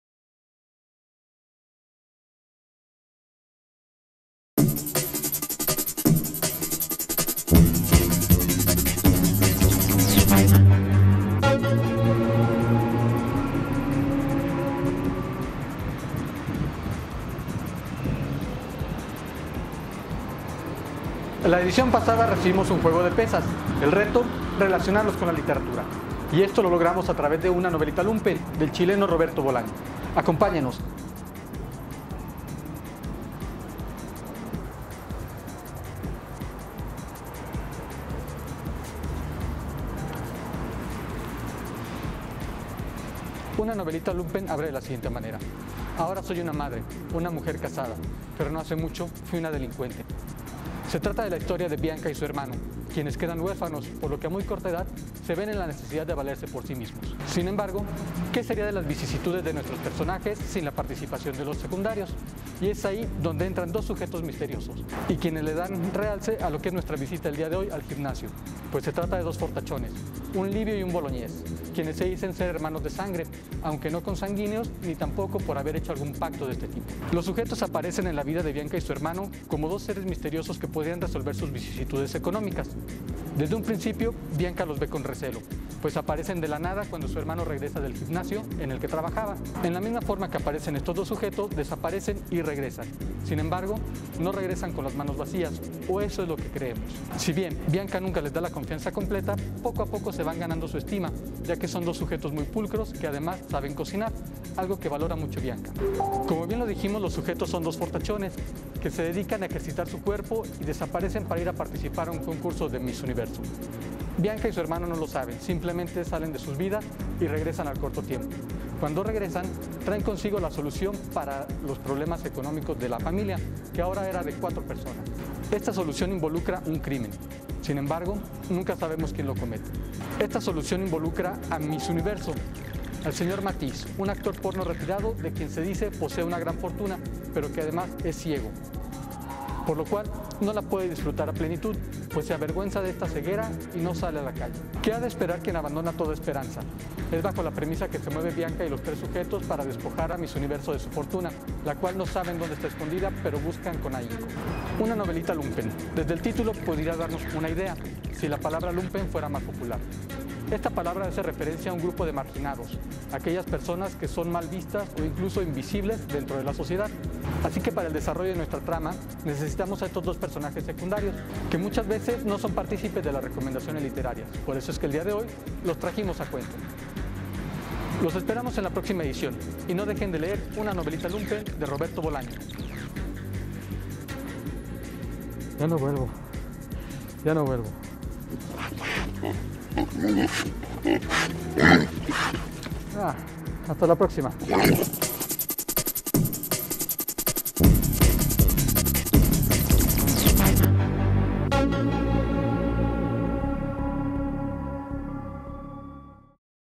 Una Noelita Lumpen | Librero Vacío
Cultura📚 | El libro de la semana: Una Novelita Lumpen, libro de 2002 del escritor chileno, Roberto Bolaño.